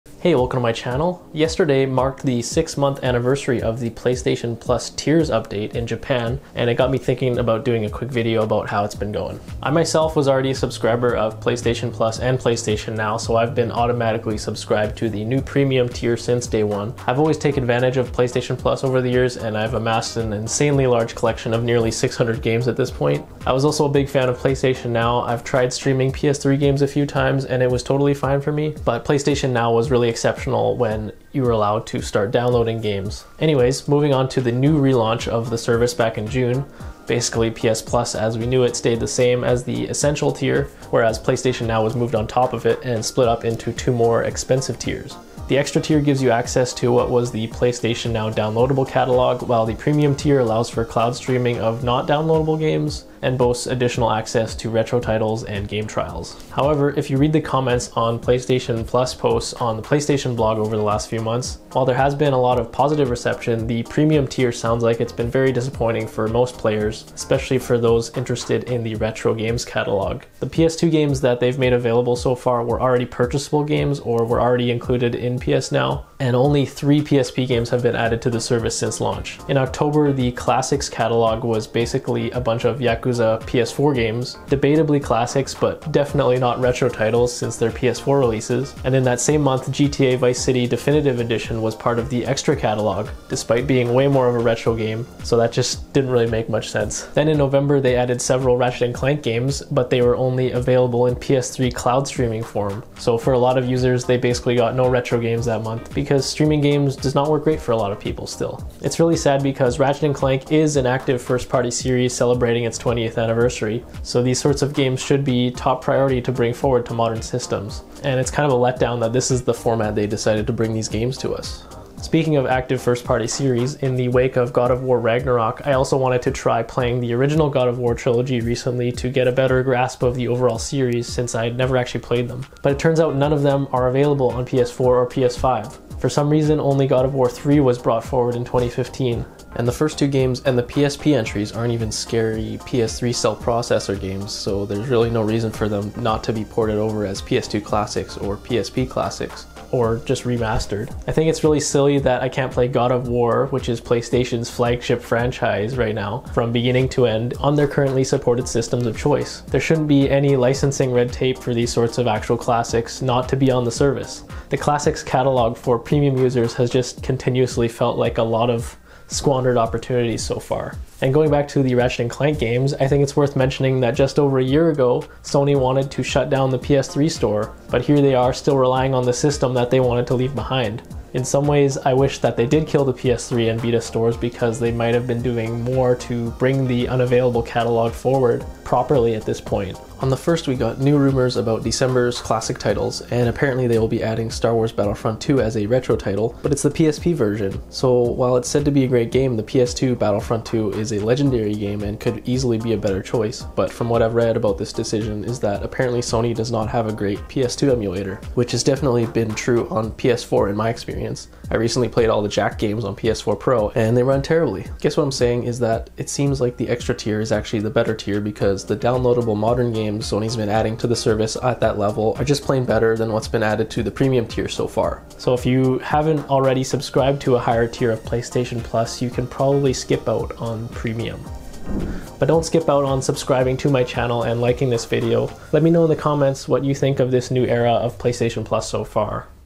PS Plus Tiers 6 Month Later
It's been 6 months since PS Plus added a tiered system, how has the Premium content been so far?